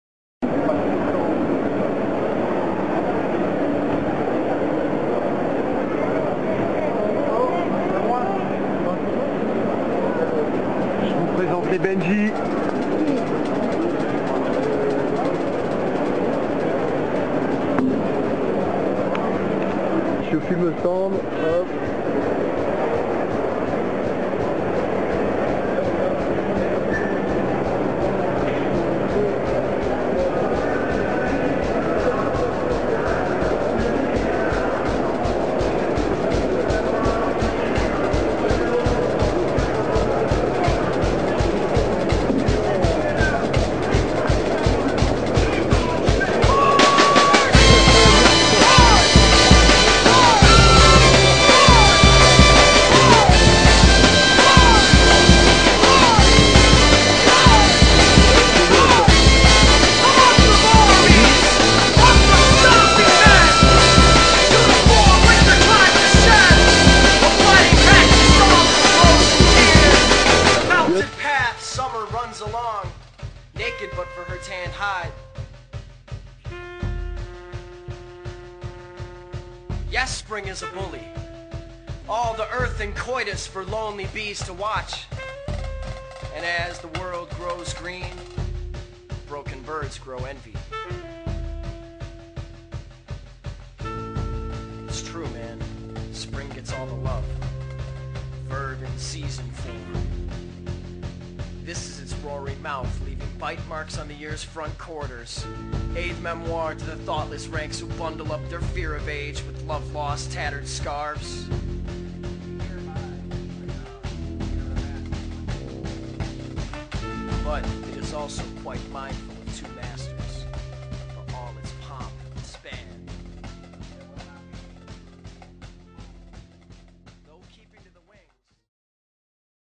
Deriveur RRS
L'équipe Gehull dévoile le deriveur benji RRS au Salon Nautique de Paris
A voir absolument lol..